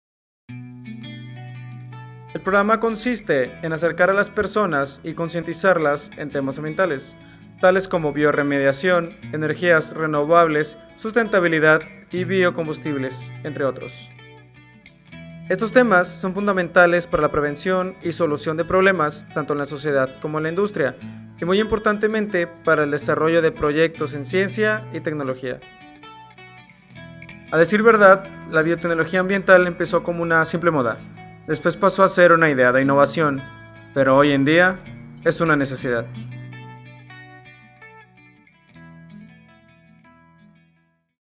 Diplomados en Biotecnología Ambiental (Monterrey, Mayo 2014)
Diplomados en Biotecnología Ambiental a realizarse en la ciudad de Monterrey, Nuevo León, México los días 9 y 10 de Mayo de 2014.

Cede: Hotel Antaris Cintermex